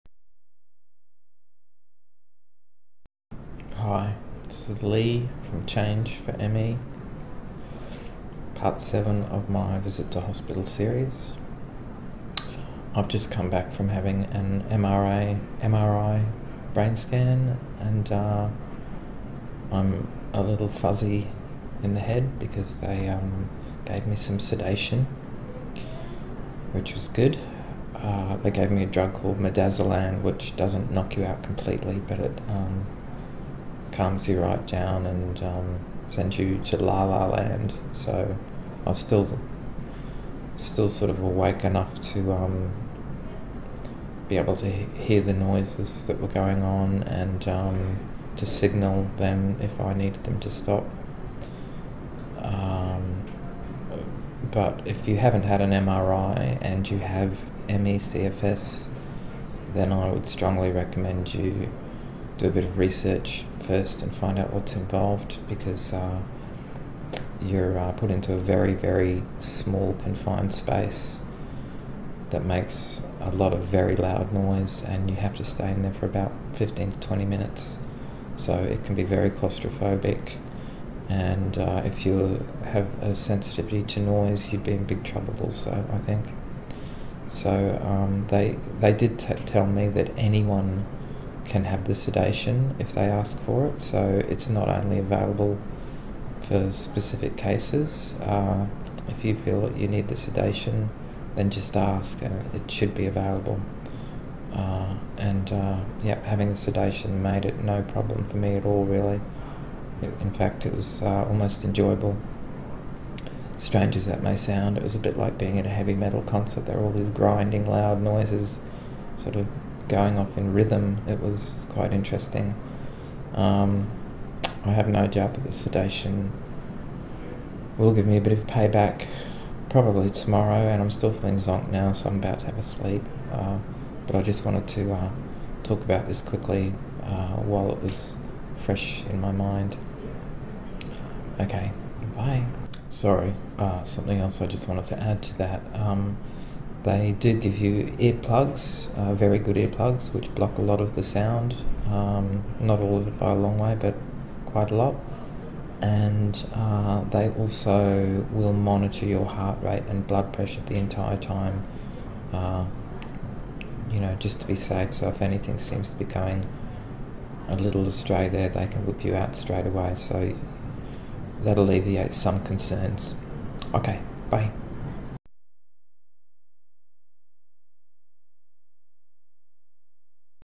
Change for ME. Visit to hospital Part. 7.
Lee co-founder of Change for ME Australia suffers with acute ME/CFS. He is house-bound, so in order to get the tests and treatment he requires, Lee has been admitted to hospital, from where he is going to share his experience with us. This is part seven of Lee's video journal from hospital.

In this part Lee discusses his MRI scan and the potential issues people with ME/CFS may encounter.

Change for ME Australia